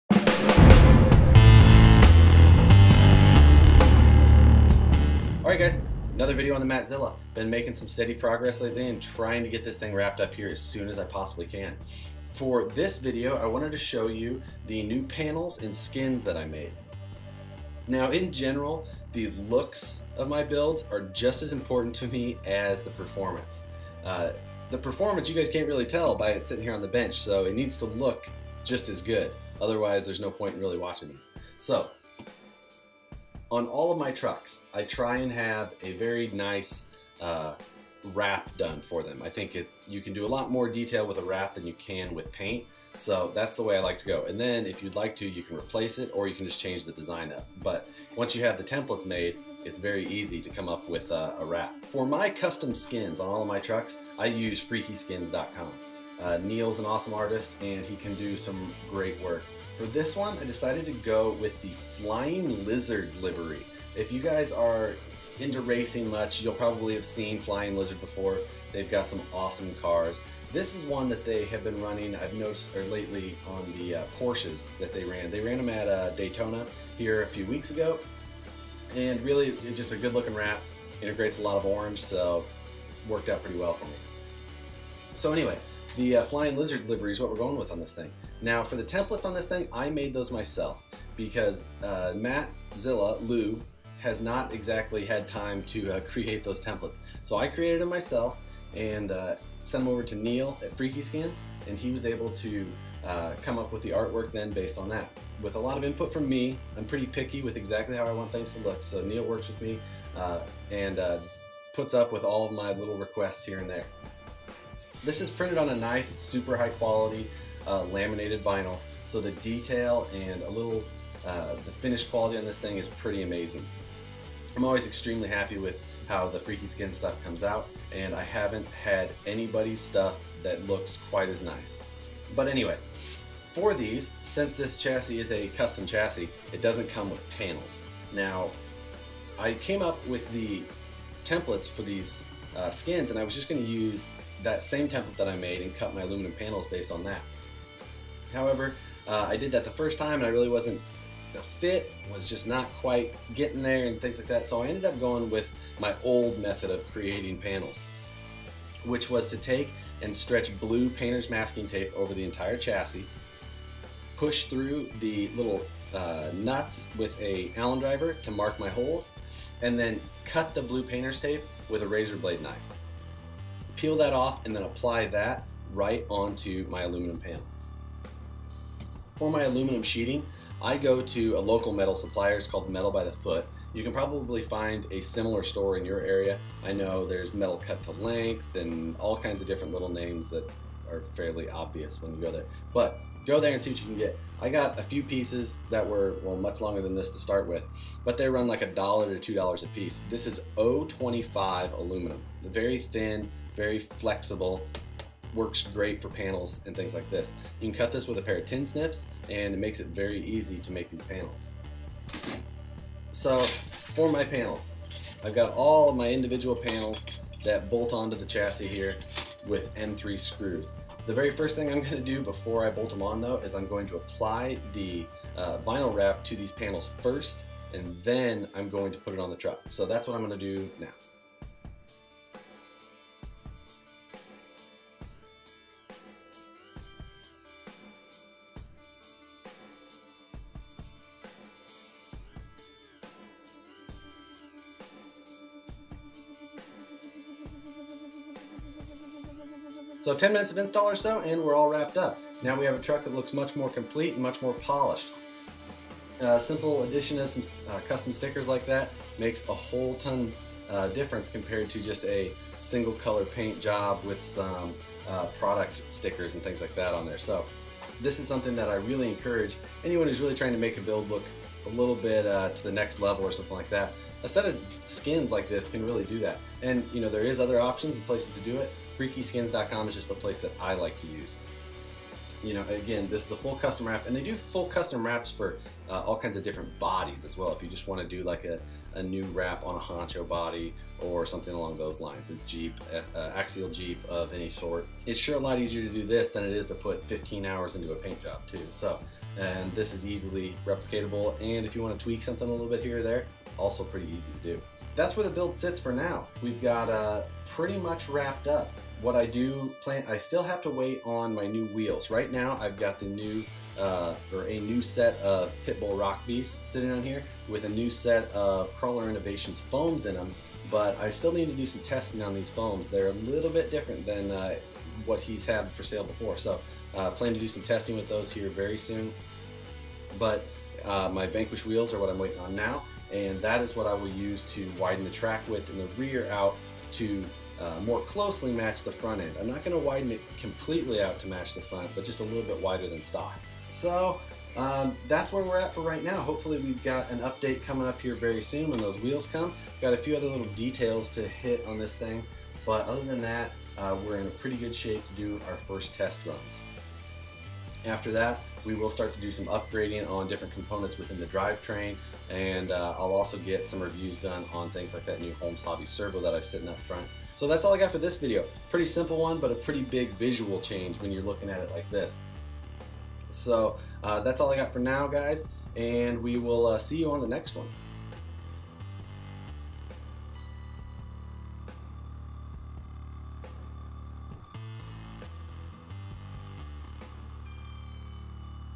Mattzilla Ultra4 - FreqEskinZ Wrap - Episode 5
The truck is really coming together! A custom wrap makes a huge impact on the final appearance and is one of my favorite parts of a build! I go to Freqeskinz.com for all my wraps. The quality is much higher than the other RC brands I have used before.